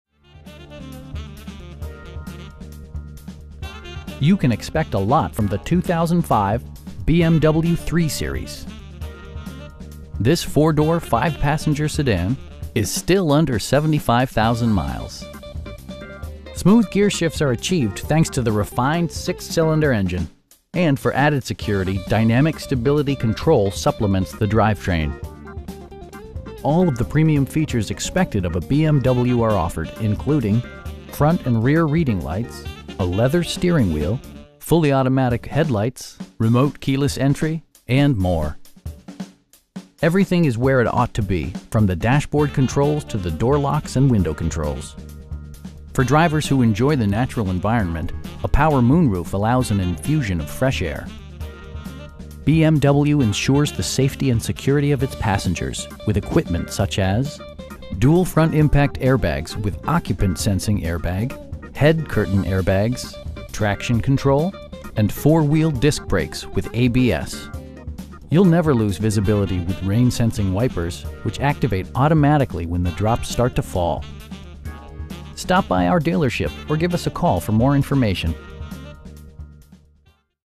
2005 BMW 325i in Portland, OR 97201
BMW Portland
2001 SW Jefferson St

You can expect a lot from the 2005 BMW 3-Series. This 4 door, 5 passenger sedan is still under 75,000 miles! Smooth gearshifts are achieved thanks to the refined 6 cylinder engine, and for added security, dynamic Stability Control supplements the drivetrain. All of the premium features expected of a BMW are offered, including: front and rear reading lights, 1-touch window functionality, a leather steering wheel, front bucket seats, fully automatic headlights, remote keyless entry, and more. Everything is where it ought to be, from the dashboard controls to the door locks and window controls. For drivers who enjoy the natural environment, a power moon roof allows an infusion of fresh air. BMW ensures the safety and security of its passengers with equipment such as: dual front impact airbags with occupant sensing airbag, head curtain airbags, traction control, brake assist, ignition disabling, and 4 wheel disc brakes with ABS. You'll never lose visibility with rain sensing wipers, which activate automatically when the drops start to fall. We have the vehicle you've been searching for at a price you can afford. Stop by our dealership or give us a call for more information.